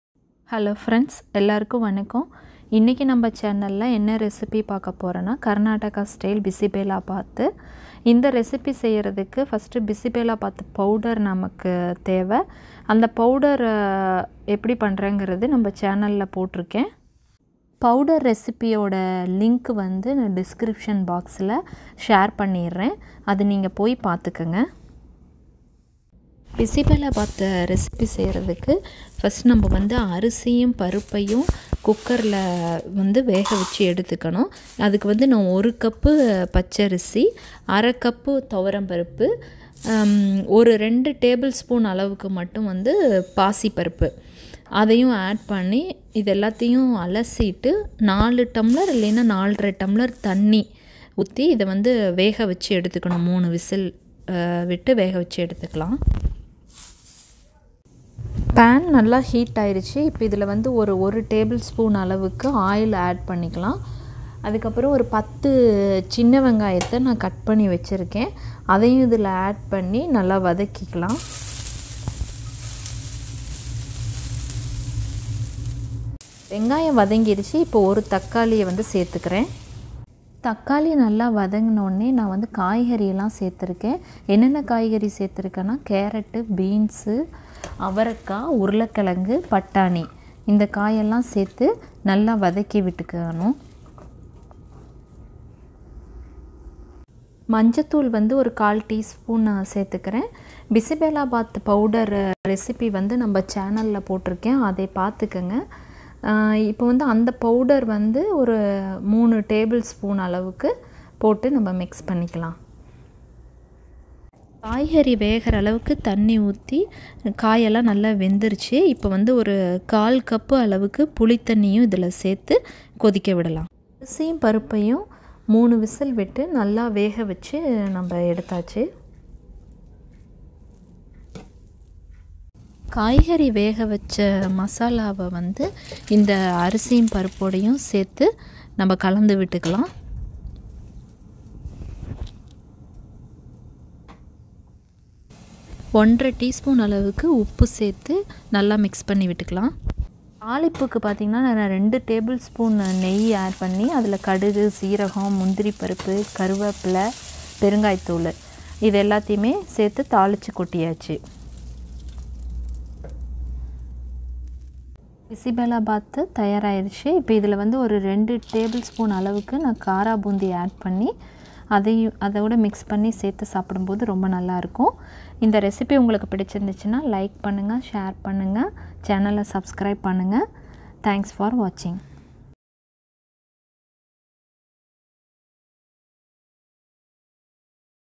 Karnataka Style Bisibelebath | பிசிபேல்லாபாத் | Sambar satham | சாம்பார் சாதம்
Karnataka | Style | Bisibelebath | பிசிபேல்லாபாத்
Sambar | Satham | சாம்பார் | சாதம்

Rice-1cup
Toor dal-1/2cup
Moong dal-2tbsp
Bisibelebath powder-3tbsp
Mixed vegetables
Small onion
Tomato-1
Tamarind juice-1/4cup